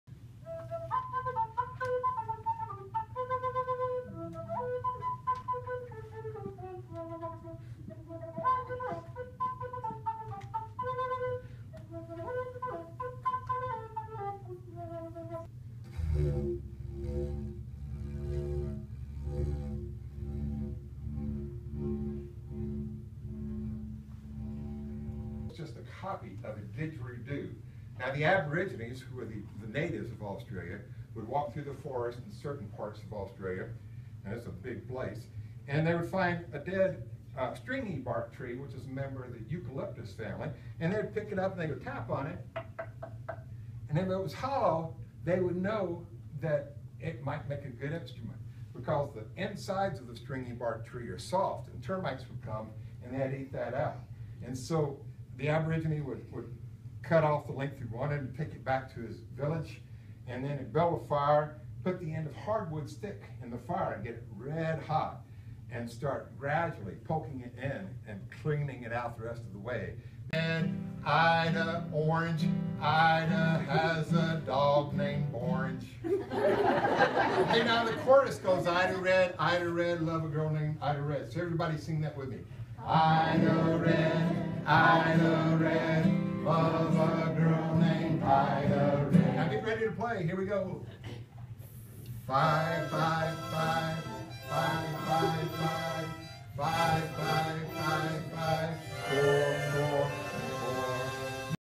Seay performs at Kimball Library
David Seay of Avoca gave his unique presentation of various folk instruments, both usual and unusual, Sunday afternoon at the Kimball Public Library. The sometimes interactive presentation gave those who attended an insight on the history of various folk instruments and how they have been used. Seay also gave a beginning harmonica lesson to anyone who wanted it.